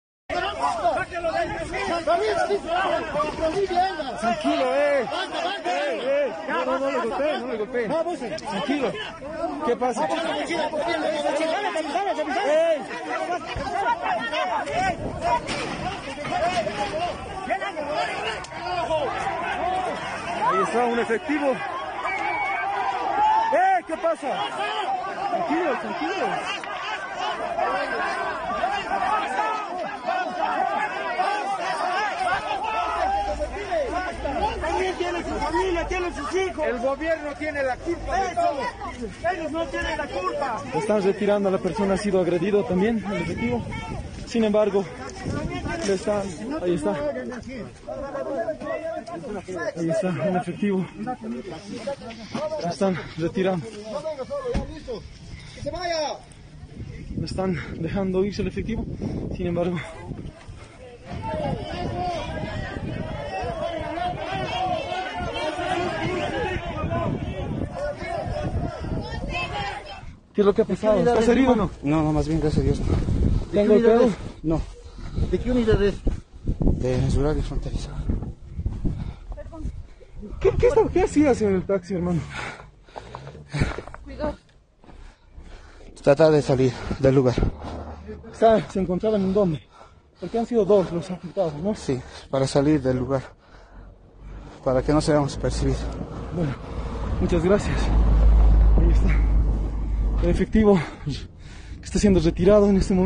Cocaleros golpean a dos policías
Dos policías, uno de ellos de civil, recibieron una golpiza de parte de los cocaleros en medio de la toma de la sede de Adepcoca.

Video: Erbol